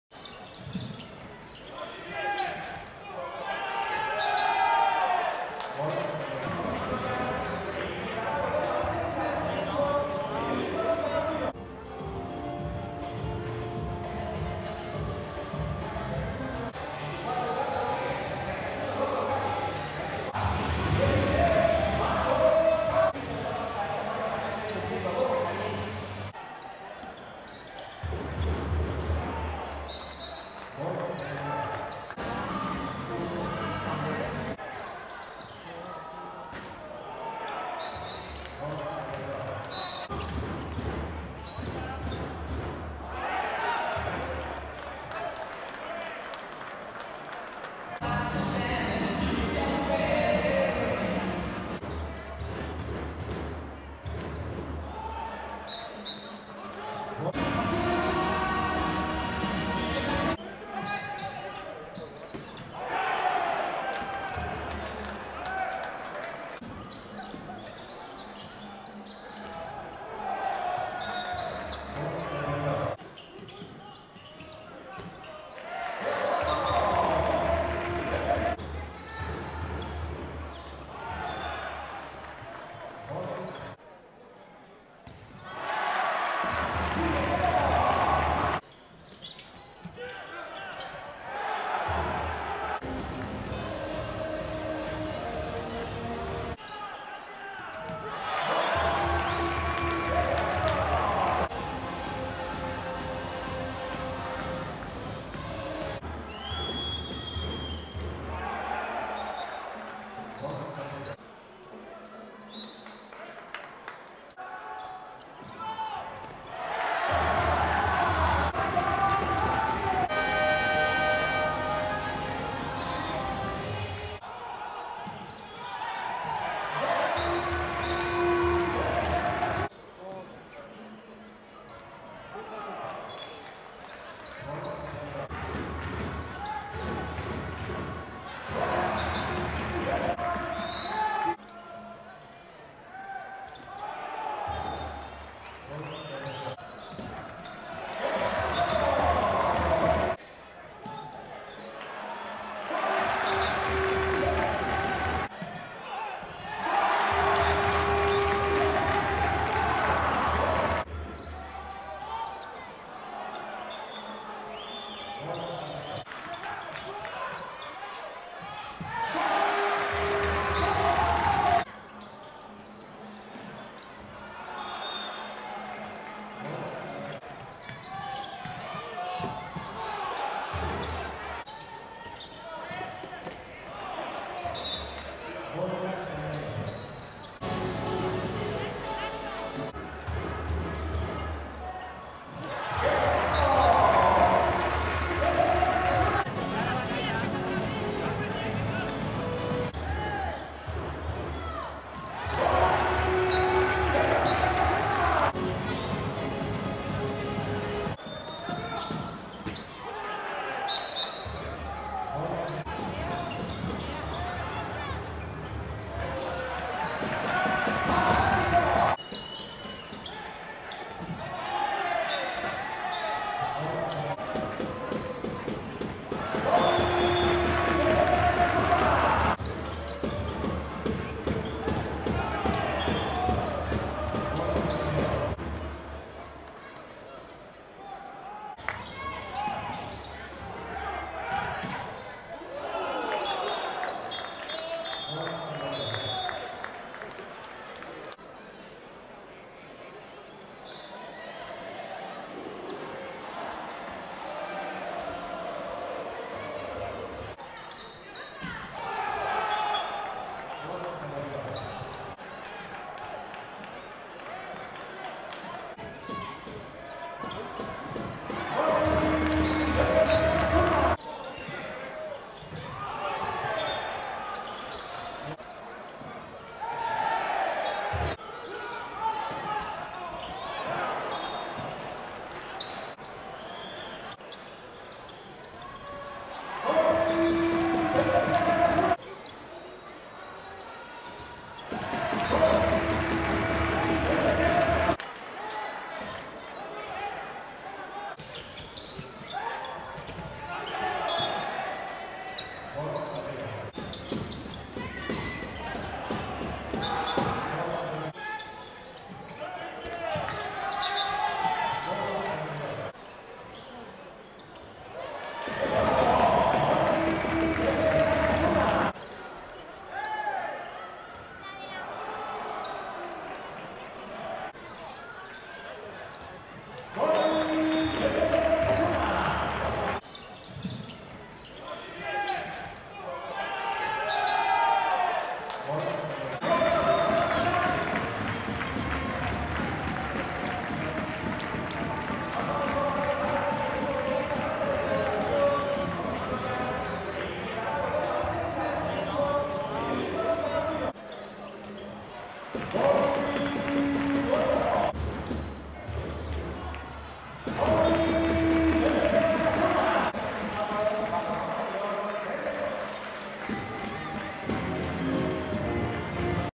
32ª Liga Sacyr ASOBAL J22: Helvetia Anaitasuna - BM. Benidorm 24-32
Partido disputado en Pamplona perteneciente a la 22ª Jornada  de la Liga Sacyr ASOBAL Temporada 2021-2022